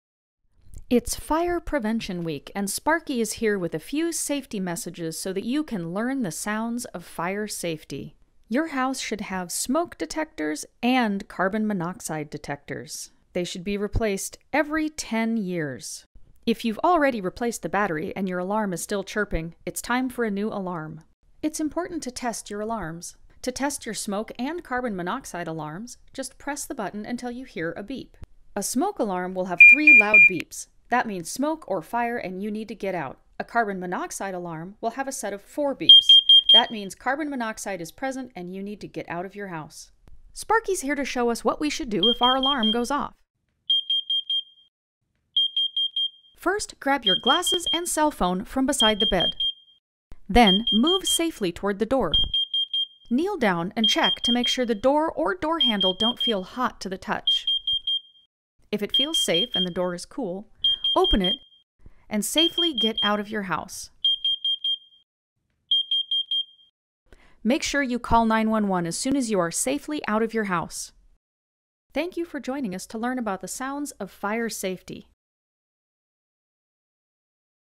Sounds of Fire Safety
Smoke and carbon monoxide detectors make different sounds. Do you know how to tell the difference? When is a beep just a reminder, and when should you call 911? Find out with Sparky!